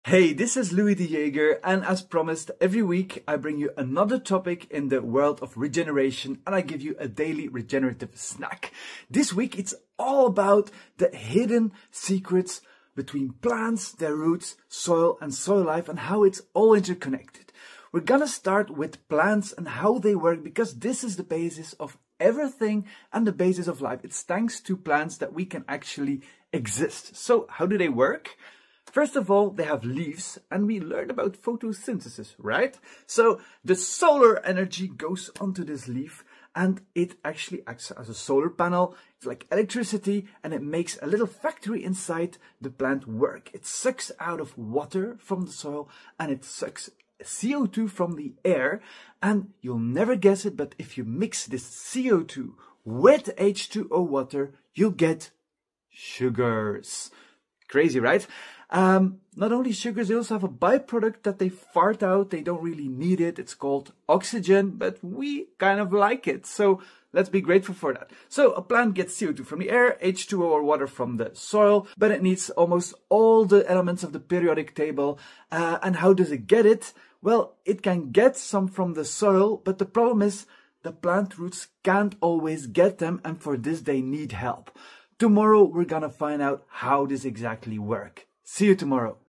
How do plants work 🌱
This week, I'm bringing you a new series with a daily regenerative snack:
All about the hidden secrets between plants, their roots, and soil life. 🌍

Let’s start at the beginning: How do plants actually work?

Plants are like living solar panels.

They capture sunlight, pull CO2 from the air and water from the soil, and magically produce sugars (and a by-product they don't even need: oxygen — pretty important for us!).

But here's the kicker: plants also need almost every element of the periodic table. They can't always reach those nutrients on their own. They need help.